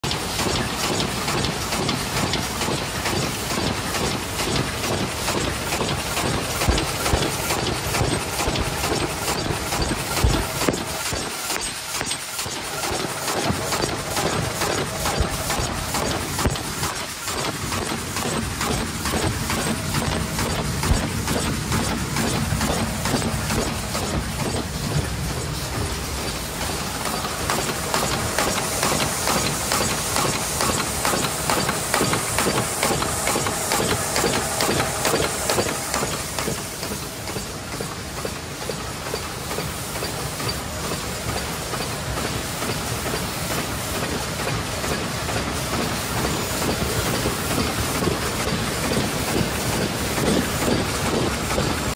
2024 new model working in customer factory NewTop-FW125S Servo paper bowl machine 135pcs/min
Manufacturer: Zhejiang New Debao Machinery Co.,Ltd.